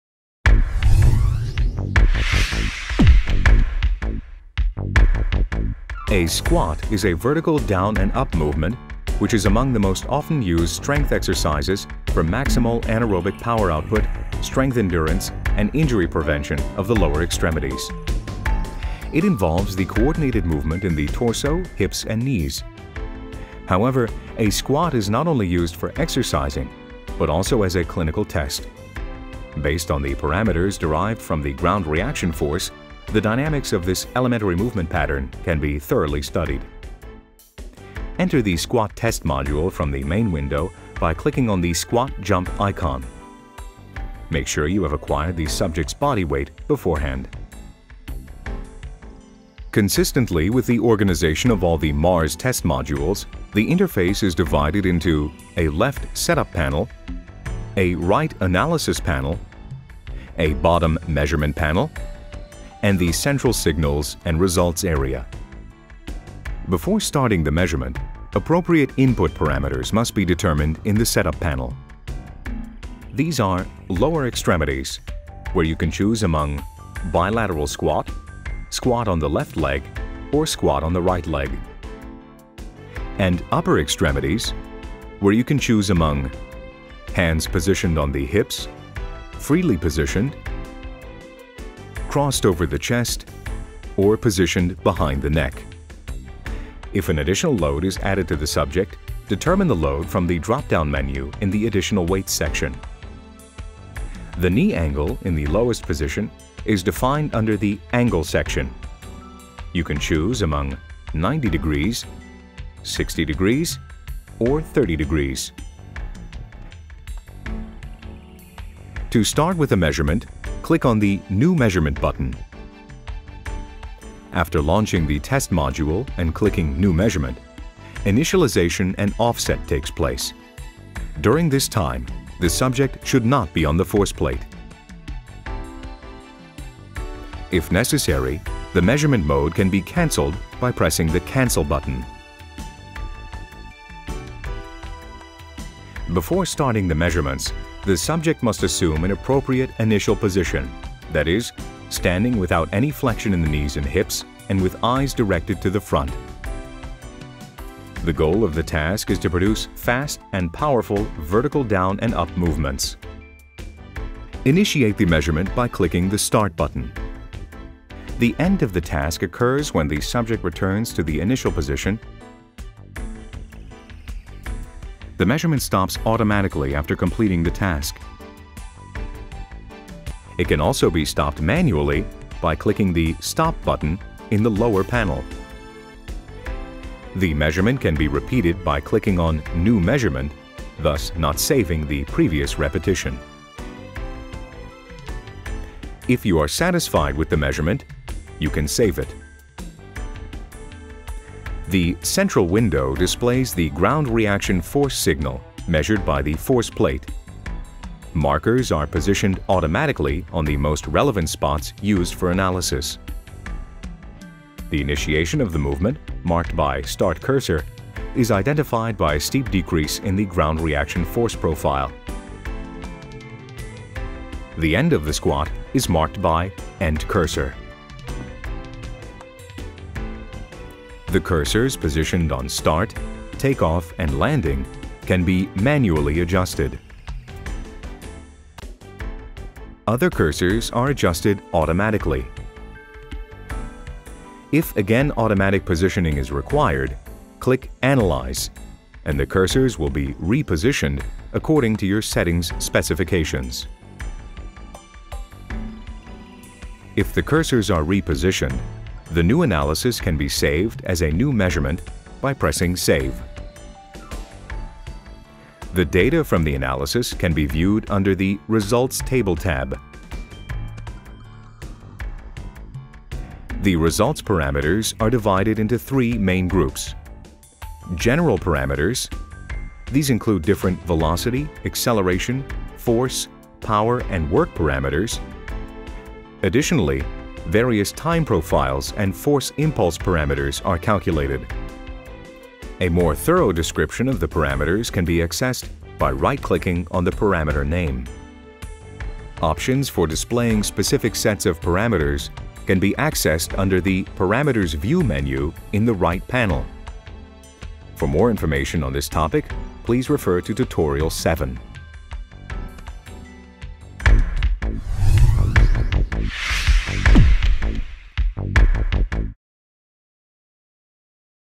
Why Squat is performed and how the performance is measured
A squat is a vertical down-and-up movement of the body without jumping. It serves as a performance and clinical test to determine anaerobic power output, strength endurance and injury risk of the lower extremities. Based on the parameters derived from the ground reaction force, dynamics of this elementary movement pattern can be thoroughly studied – learn more in the according to Kistler MARS module video tutorial.